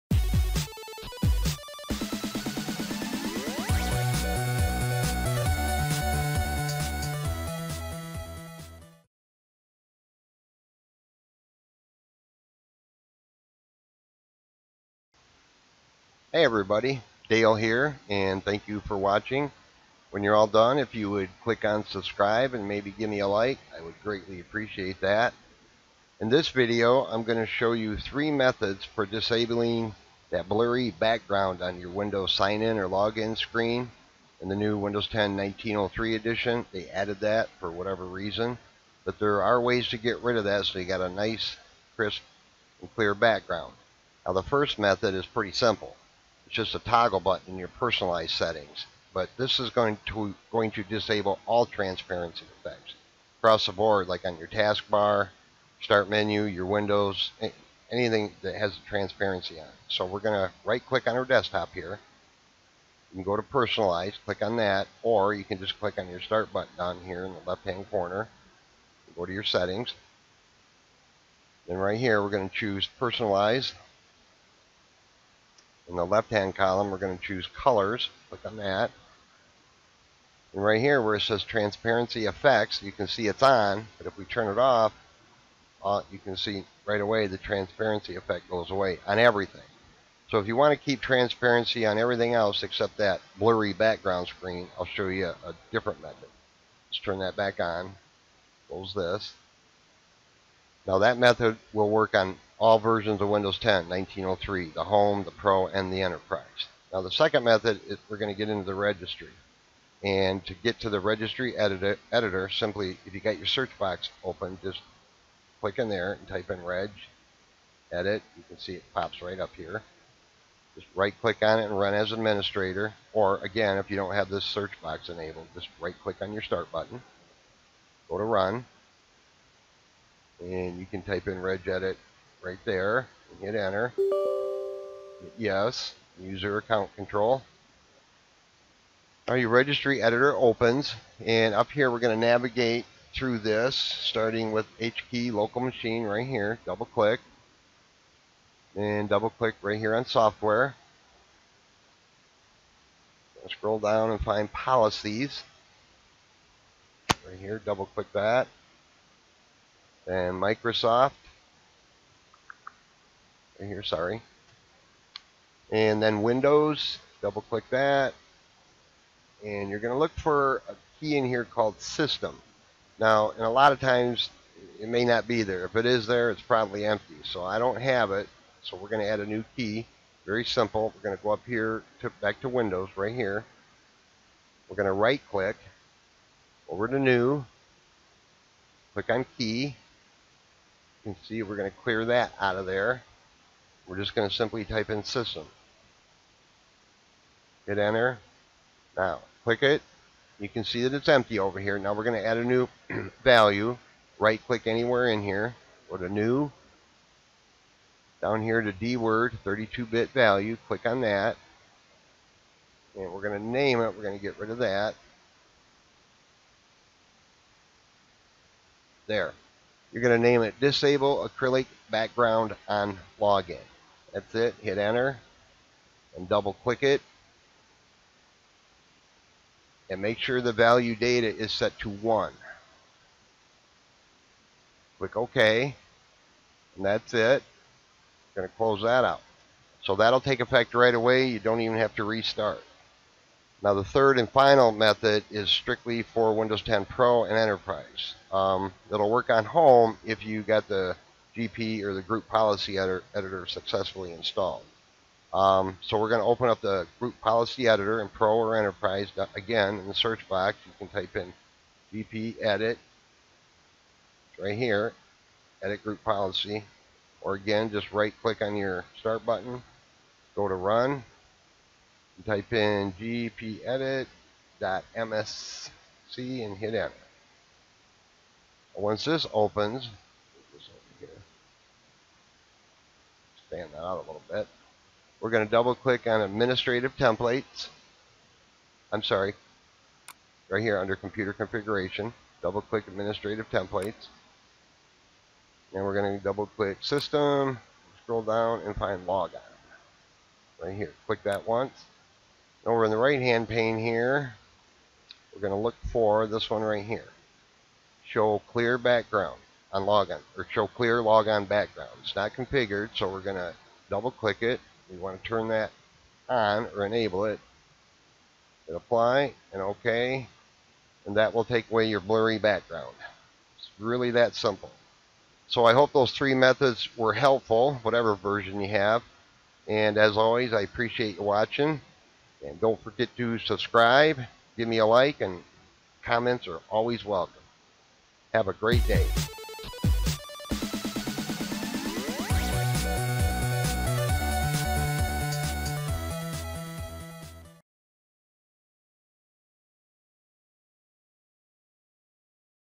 Disable Blurry Background / Transparency on Windows 10 Logon Screen
How to disable blurry background on Windows 10 Logon or Sign in screen (1903 Edition) Windows 10 Home, Pro or Enterprise. 3 Methods